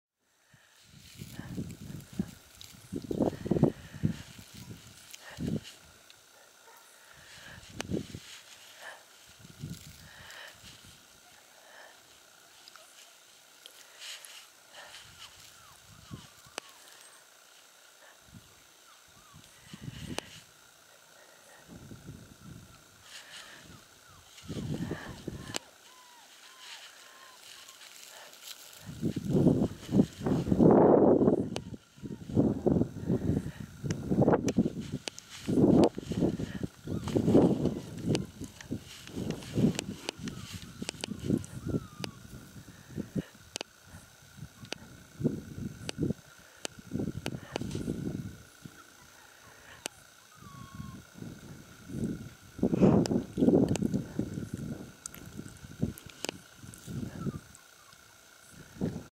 Mesa and the egg toy. Jan 31, 2022
Mesa loves her egg toy. She chews it to make it crackle and squeak. Nightshades Border Collies. January 30, 2022.